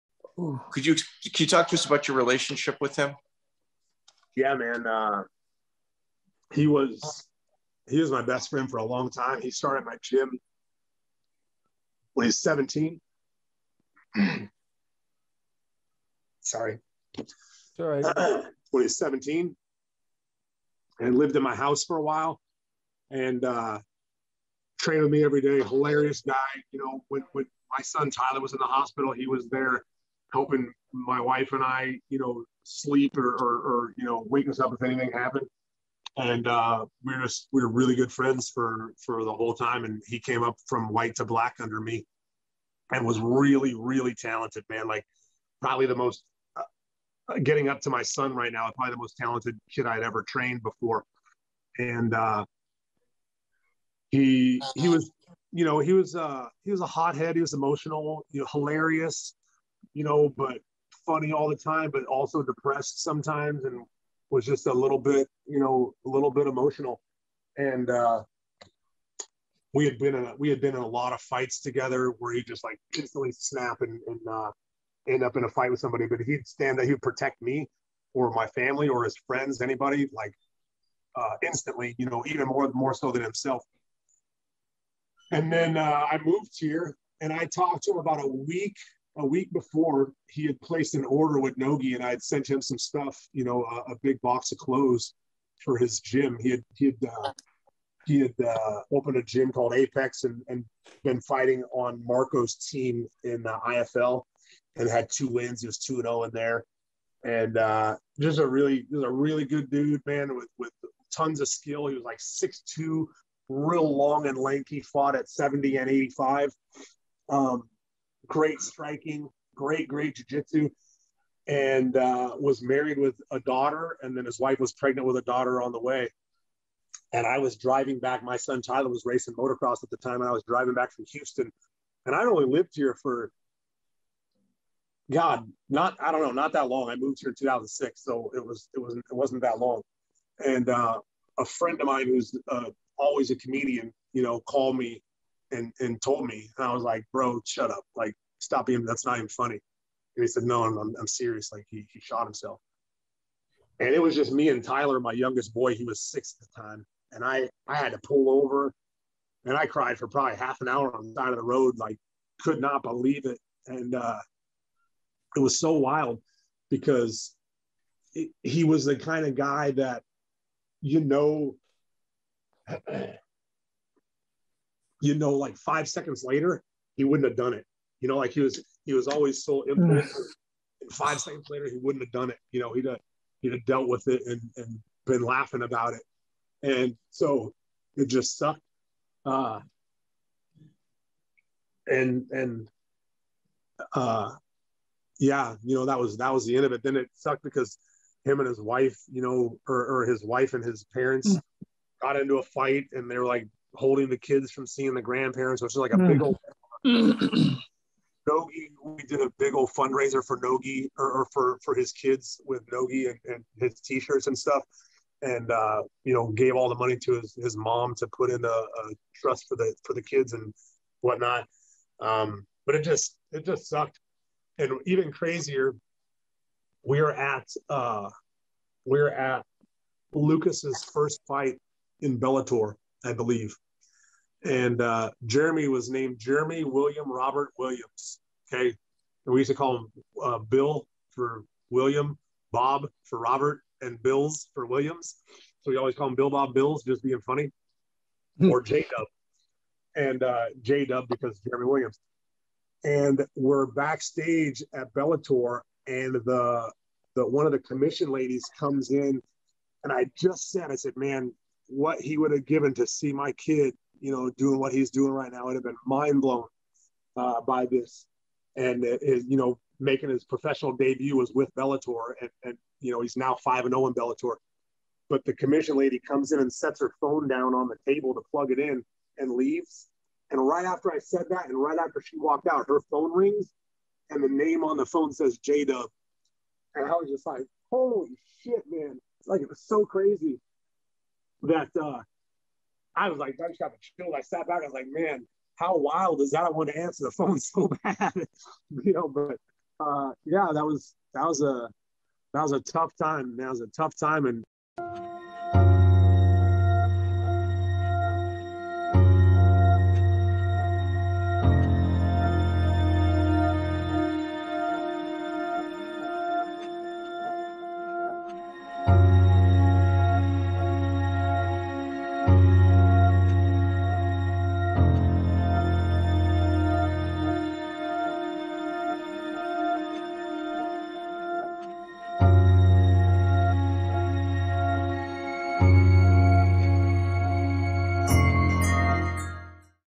R.I.P. Jeremy Williams (June 1st, 1979 - May 5th, 2007)
In this short clip, Chris Brennan remembers his student and friend Jeremy Williams. Gone, but not forgotten, Jeremy Williams  (June 1st, 1979 - May 5th, 2007).

Thanks for listening to R.I.P. Jeremy Williams (June 1st, 1979 - May 5th, 2007)

Follow the LOP team on Instagram
Chris - @chrislightsoutlytle
Mike - @mikedavis632
Miguel - @iturratem